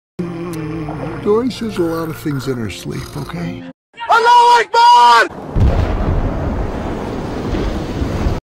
dory the terrorist
Dory says a lot of things in her sleep